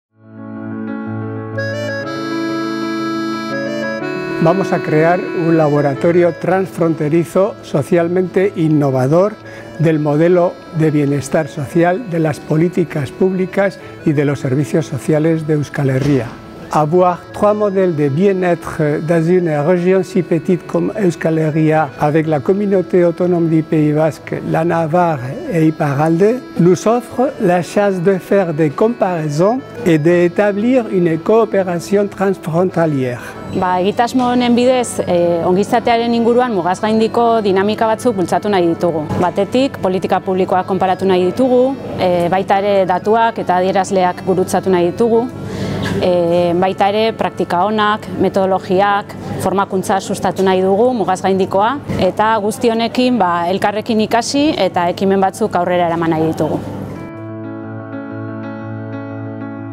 Ongizate Ereduak Euskal Herrian
Ongizate Ereduak Euskal Herrian-Modelos de Bienestar en Euskal Herria-Modèles de Bien-étre en Euskal Herria

Vamos a crear un laboratorio transfronterizo, socialmente innovador, del modelo de bienestar social, de las políticas públicas y de los servicios sociales de Euskal Herria.

Avoir trois modèles de bien-être dans une région petite comme est Euskal Herria avec la Communaute Autonome du Pays Basque, la Navarre et Iparralde, nous offre la chase de faire de comparaison et de établir une cooperation tranfrontalier.

Ba egitasmo honen bidez, ongizatearen inguruan mugaz gaindiko dinamika batzuk pentsatu nahi ditugu. Batetik politika publikoak konparatu nahi ditugu baita ere datuak eta adierazleak gurutzatu nahi ditugu.
Baita ere praktika onak, metodologiak, formakuntza sustatu nahi dugu mugaz gaindikoa eta guzti honekin elkarrekin ikasi eta ekimen batzuk aurrera eraman nahi ditugu.

Eusko Ikaskuntzaren XVIII. Kongresuaren liburuaren eta bere lan-programen aurkezpena  / Presentación del libro del XVIII Congreso de Estudios Vascos y los programas de trabajo de Eusko Ikaskuntza

Kutxa Kultur Plaza, Tabakalera, Donostia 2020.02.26